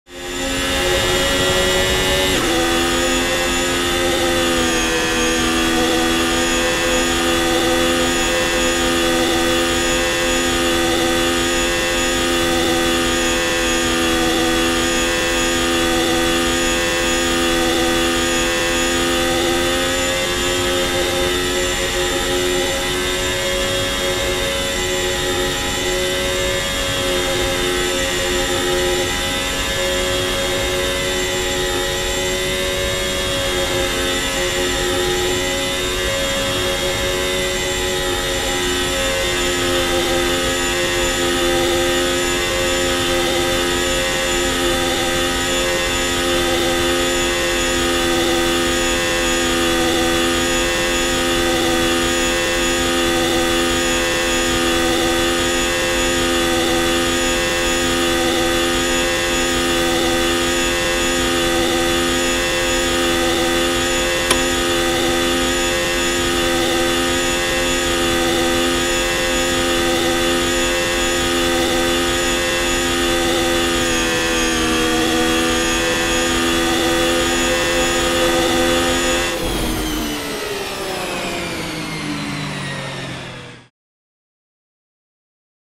Spa but the whole lap is FLAT OUT
Ride onboard for a completely Flat out Lap around Spa Francorchamps. The Bus stop Chicane was pretty challenging!